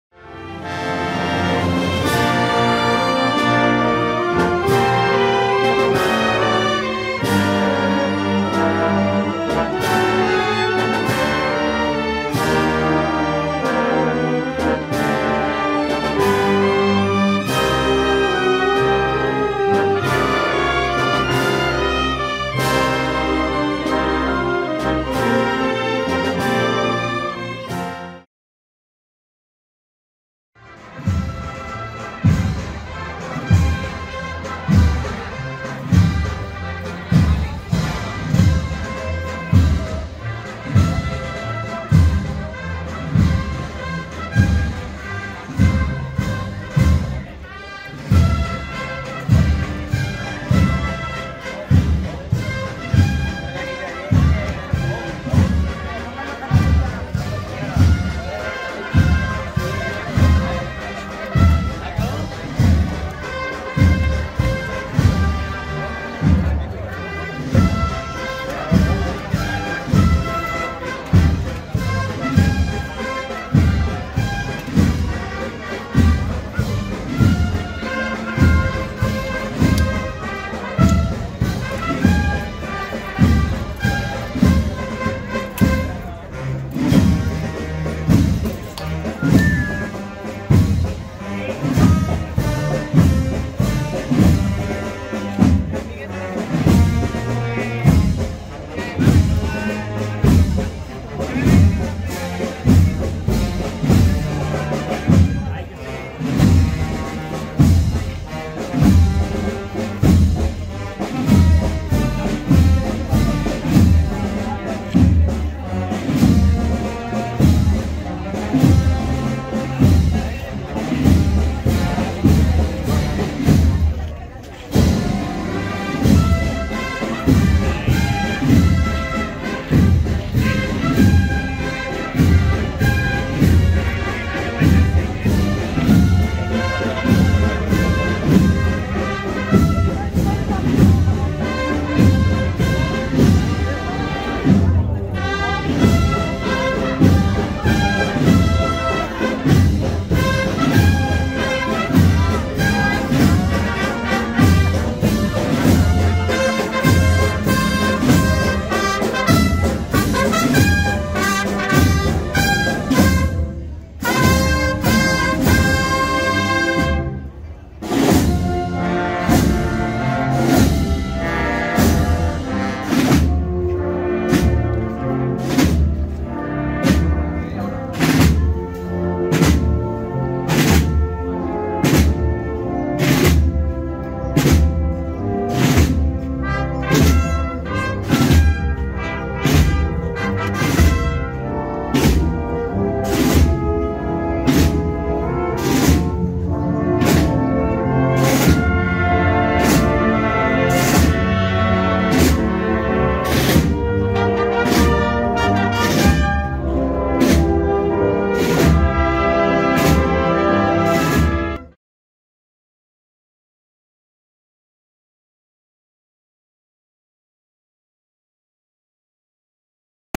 Procesión de la Virgen del Rosario Coronada | Bornos | 2023
Salida procesional de la Virgen del Rosario Coronada, Patrona de Bornos (Cádiz).

📍 Bornos (Cádiz).